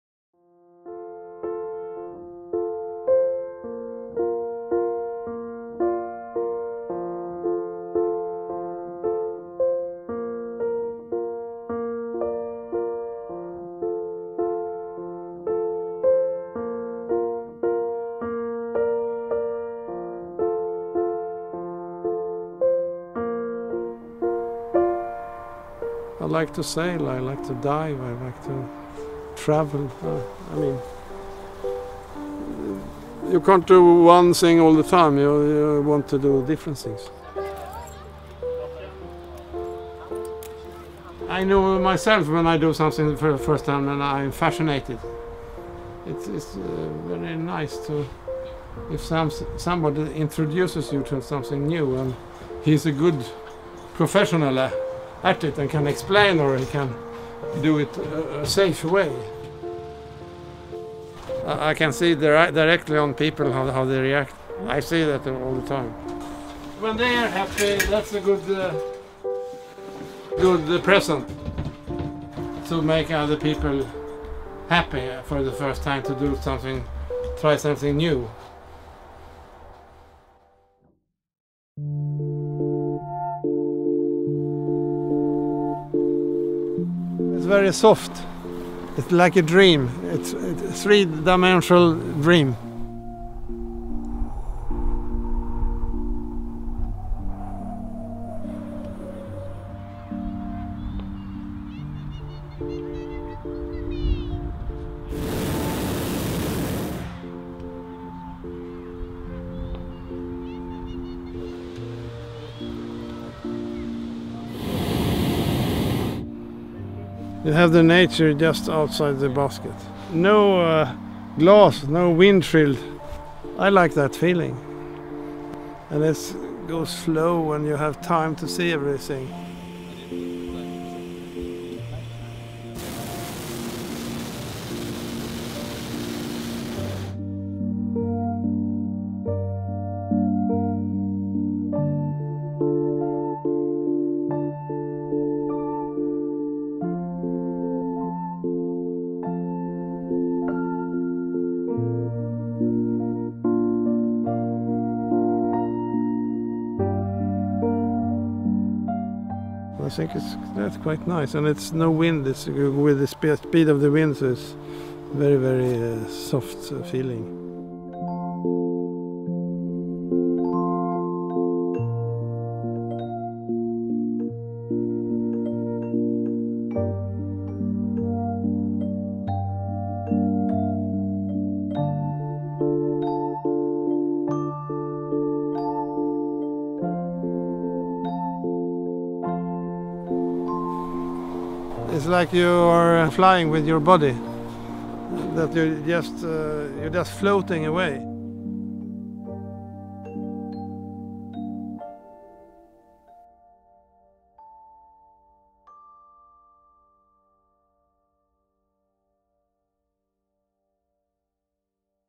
Hot air balloon in Stockholm - "No Glass, No Windshield"
Hot air balloon captain - that's a pretty cool job. When you get to fly over one of the world's most beautiful city, Stockholm - Sweden, that's even better. Still, as good as it sounds, there are not many people being able to fly balloons in Sweden any more.

On nice, warm and long-lasting summer days, Karl Gustav Silverstolpe, who runs the small and local "Ballongflyg i Stockholm" company, shares his sense of adventure, passion and expertise, and takes us for a ride up in the air. At his own pace: smooth and flowing - and from his favorite place: a simple balloon basket with no glass and no windshield.

Shot with Lumix GH4, Samyang 24mm f/1.4, Samyang 10mm f/2.8, Nikon 50mm f1.8